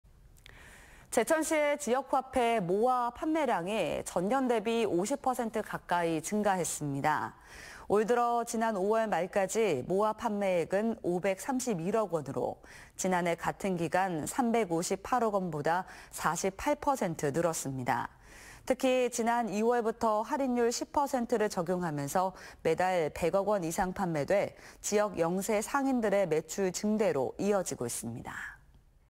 제천화폐모아판매량50%급증
* 제천화폐 '모아' 판매액 지난해보다 48% 증가 *